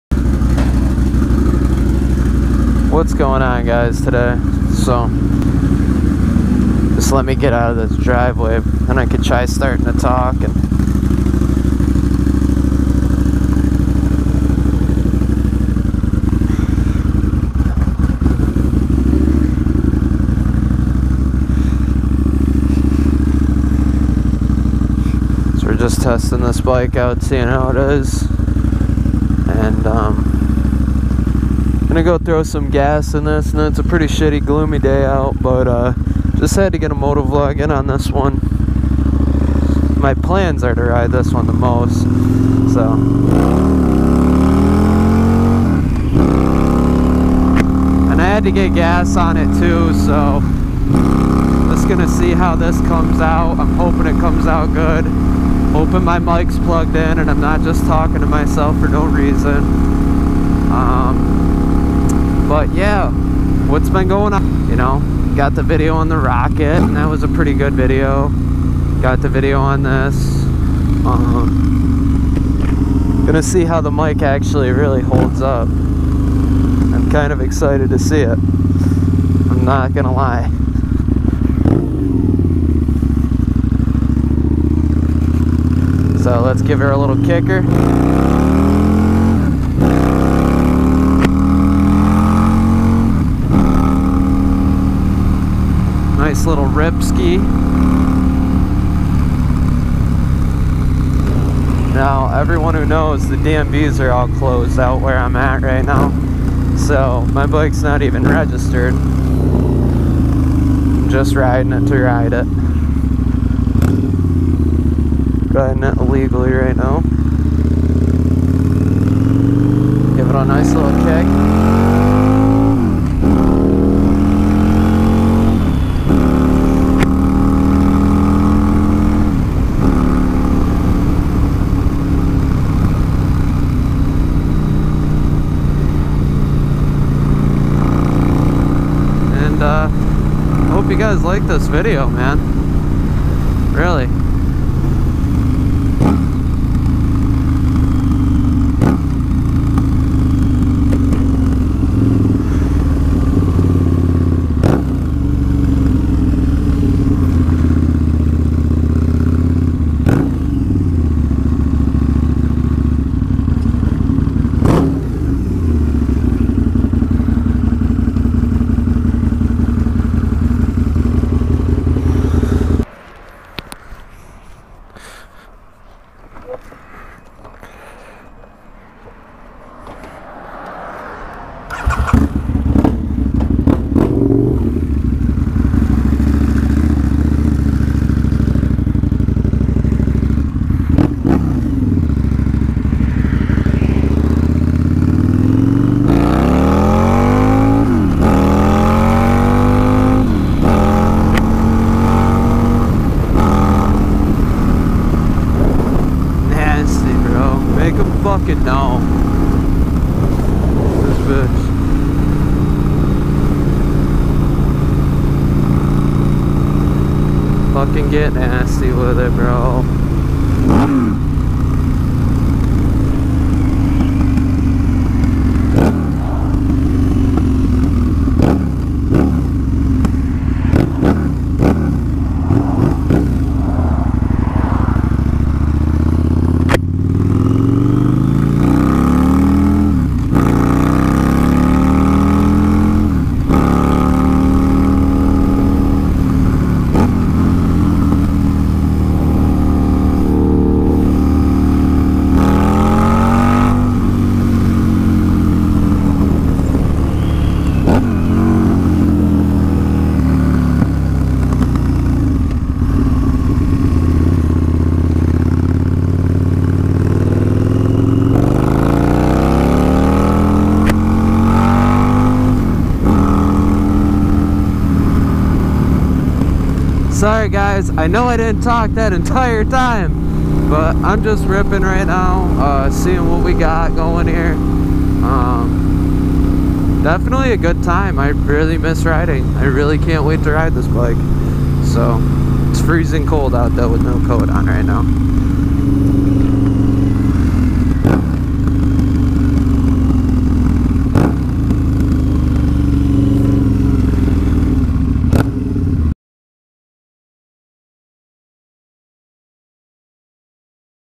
Taking the Vrod Out
In the video I take the vrod out for a ride to test out the motovlog setup